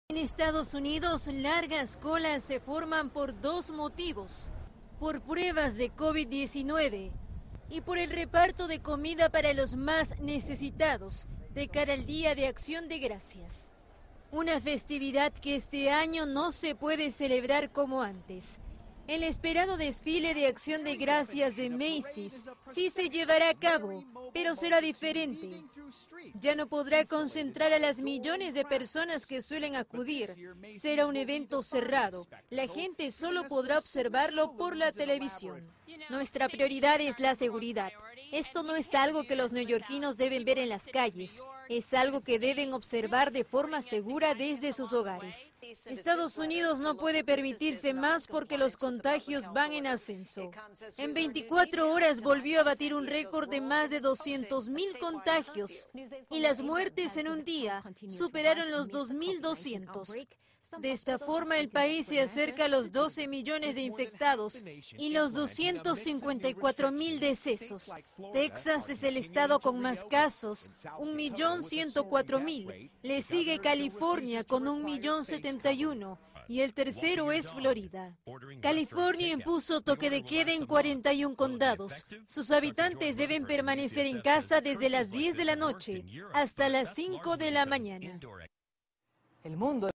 EE.UU registró más de 200 mil casos por COVID-19 en un solo día
Estados Unidos batió un nuevo récord en medio de la pandemia tras registrar más de 200 mil nuevos casos de COVID-19 en tan solo 24 horas, mientras que los decesos superaron las 2 mil 200 víctimas.

Esta situación ocurre a menos de una semana del Día de Acción de Gracias y el desfile de Macy’s, el cual no podrá concentrar a las miles de personas como en años anteriores, sin embargo, la gente podrá verlo por televisión.

Cabe mencionar que Estados Unidos, según reportes oficiales, reporta hasta la fecha casi 12 millones de contagios y 254 mil 297 muertes. De este total de infectados, Texas registra la mayor cantidad, le siguen California y Florida.